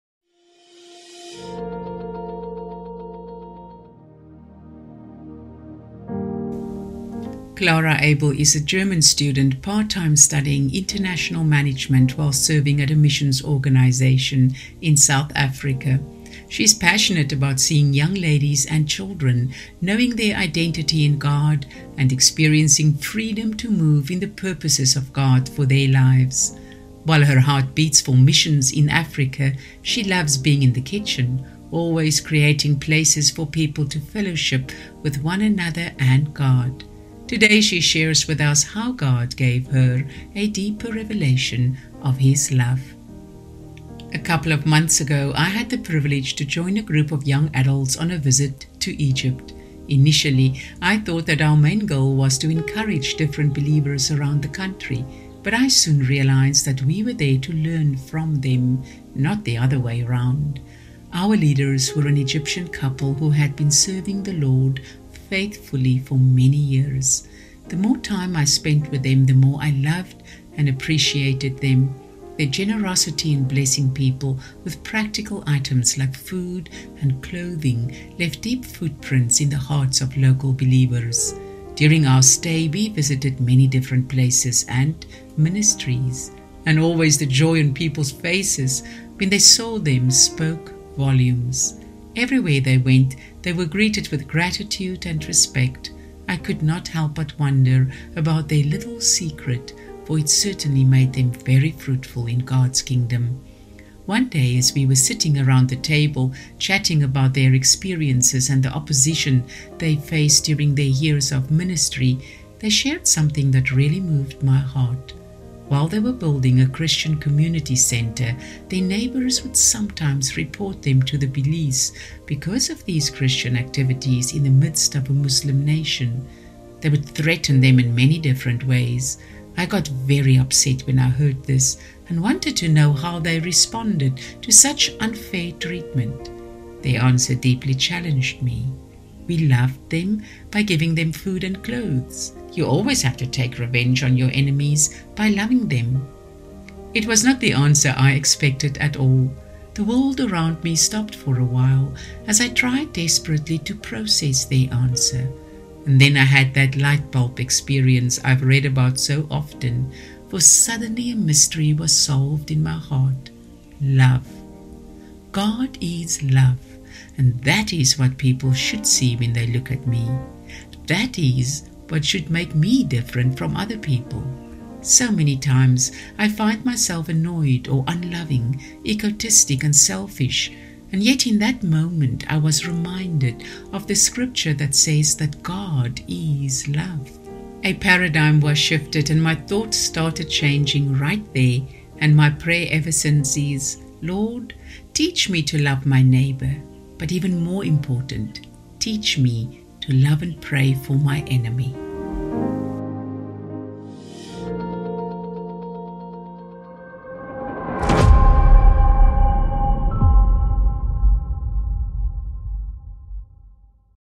A Deeper Revelations Of God's Love - Suria Scholtz
Clara Abel is a German student part time studying International Management while serving at a missions organization in South Africa. She is passionate about seeing young ladies and children knowing their identity in God and experiencing freedom to move in the purposes of God for their lives. While her heart beats for missions in Africa, she loves being in the kitchen, always creating places for people to fellowship with one another and God. Today she shares with us how God gave her a deeper revelation of His love!
A DEEPER REVELATION OF GOD’s LOVE
A couple of months ago I had the privilege to join a group of young adults on a visit to Egypt. Initially I thought that our main goal was to encourage different believers around the country, but I soon realized that we were there to learn from them, not the other way round. Our leaders were an Egyptian couple who had been serving the Lord faithfully for many years. The more time I spent with them, the more I loved and appreciated them. Their generosity in blessing people with practical items like food and clothing left deep footprints in the hearts of local believers. During our stay we visited many different places and ministries, and always the joy on people’s faces when they saw them, spoke volumes. Everywhere they went, they were greeted with gratitude and respect. I could not help but wonder about their little “secret” – for it certainly made them very fruitful in God’s kingdom.
One day as we were sitting around the table, chatting about their experiences and the opposition they faced during their years of ministry, they shared something that really moved my heart.
While they were building a Christian community centre their neighbours would sometimes report them to the police because of these Christian activities in the midst of a Muslim nation. They would threaten them in many different ways. I got very upset when I heard this and wanted to know how they responded to such unfair treatment. Their answer deeply challenged me: "We loved them by giving them food and clothes. You always have to take revenge on your enemies by loving them.”
It was not the answer I expected at all. The world around me stopped for a while as I tried desperately to process their answer. Then I had that lightbulb experience I have read about so often, for suddenly a mystery was solved in my heart.
LOVE! God is love and that is what people should see when they look at me. That is what should make me different from other people. So many times, I find myself annoyed or unloving, egoistic and selfish and yet, in that moment I was reminded of the scripture that says that God is love.
A paradigm was shifted and my thoughts started changing right there and my prayer ever since is:  “Lord teach me to love my neighbour, but even more important – teach me to love and pray for my enemy!”